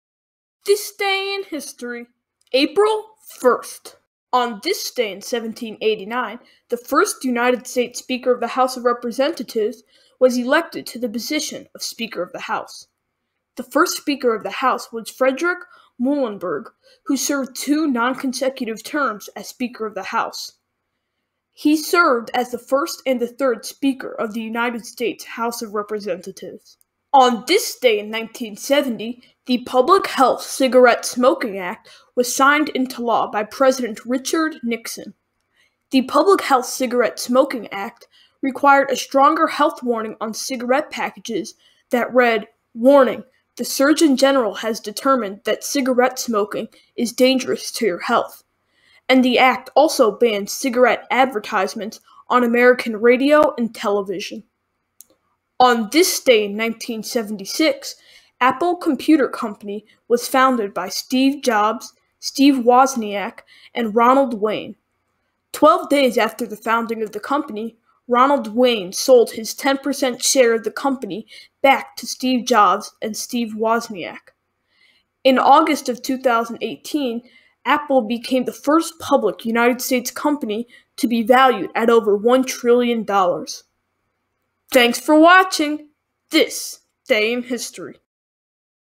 April 1 - This Day in History
This video explains historical events that occurred on April 1.